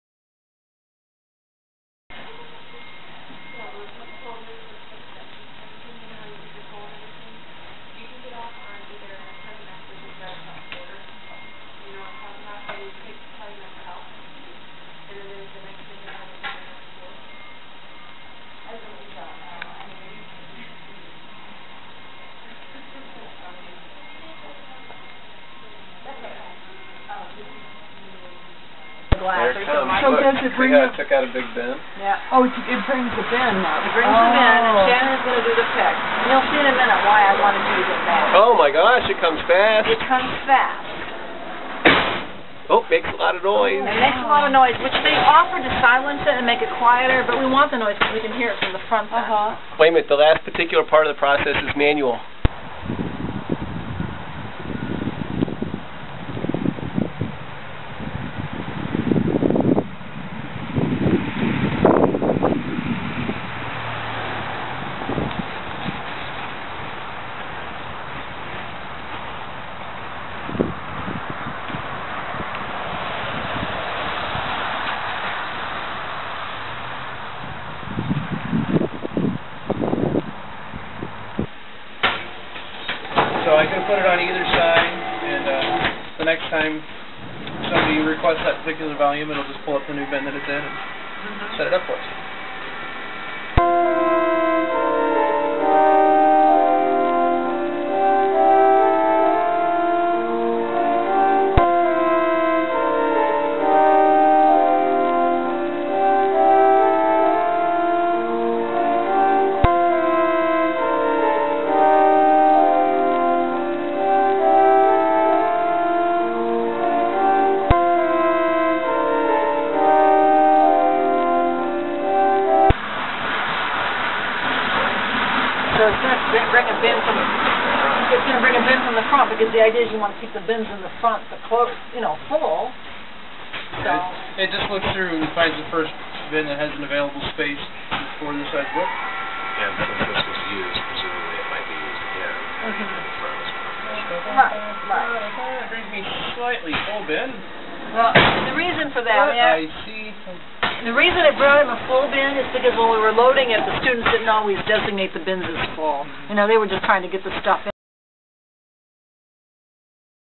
In and Around Valparaiso
This short flick shows some of the things Team DAIAD saw as we did a field trip to the Valparaiso University Library. Watch for the cool book delivering robot -- modern day closed stacks.